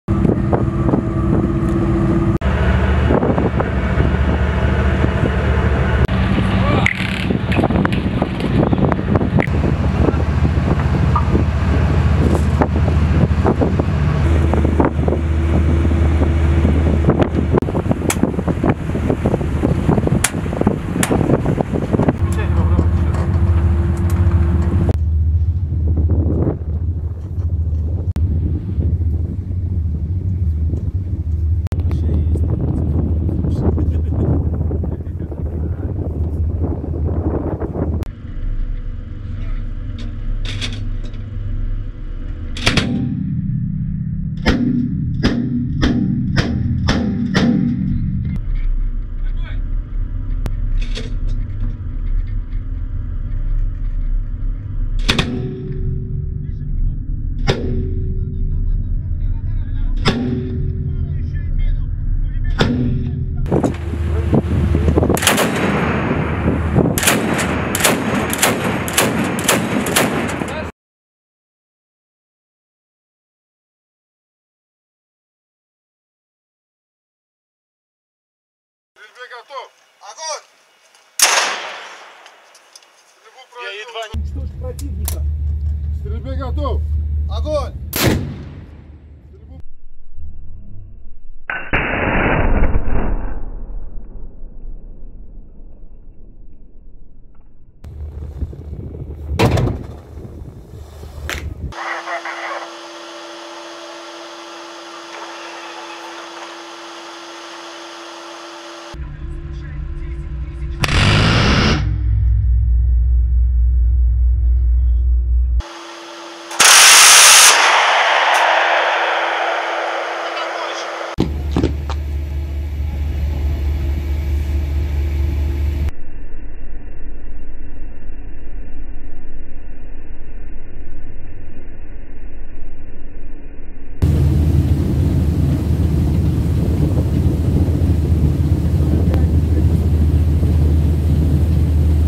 Crews of the Casspian Flotilla missile ships go to sea to conduct live-fire drills
The personnel will practice elements and standards of maritime training, tasks for mine protection of the ship, as well as for the protection and defence of the ship when parked in an unprotected roadstead.